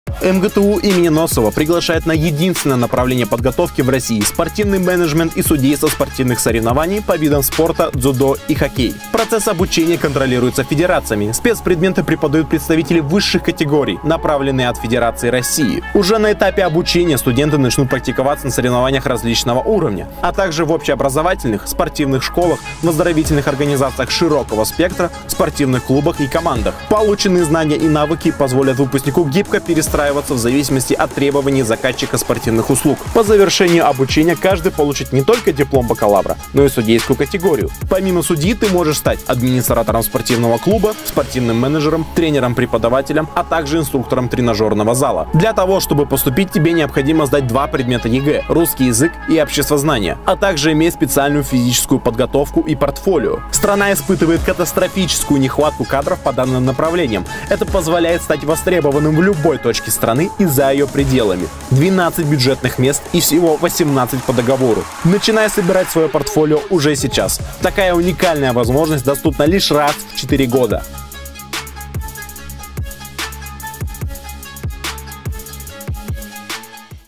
Как стать спортивным судьёй по дзюдо. Высшее образование в области судейства соревнований.
ФГБОУ ВО «Магнитогорский государственный технический университет им. Г.И. Носова» факультет физической культуры и спортивного мастерства предоставляет уникальную возможность получить образование по направлению «Физическая культура» профиль «Спортивный менеджмент и судейство спортивных соревнований», которое является единственным в России.